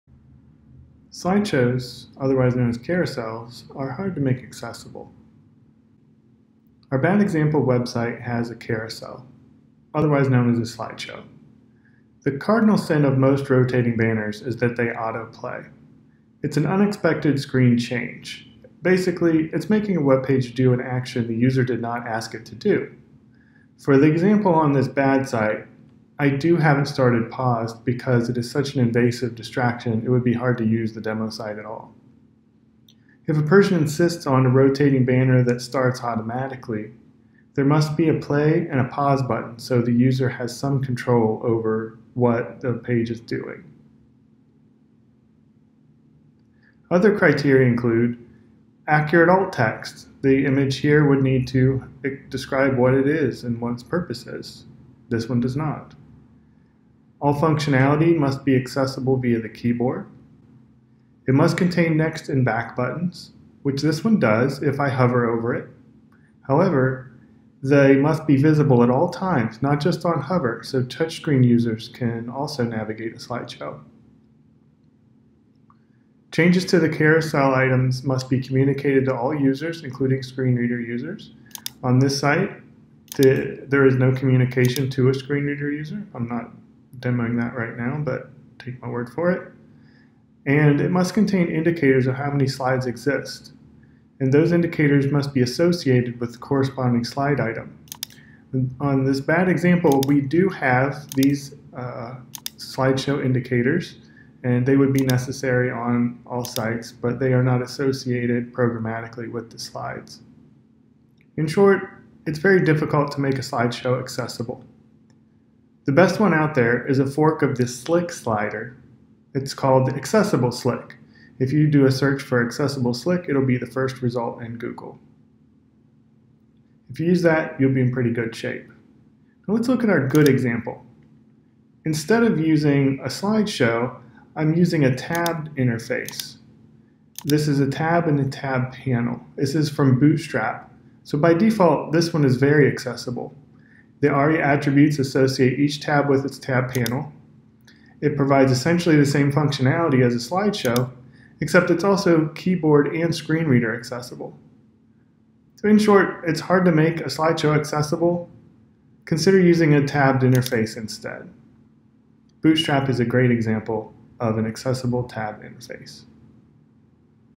Slideshow/carousel assessment
Slideshows are very difficult to make accessible. This video does not explain how to fix a slideshow. It just mentions a few requirements that a slideshow must meet.

A detailed explanation can be found in the Smashing Magazine article

The ARIA Authoring Practices Guide also has details on making a slideshow accessible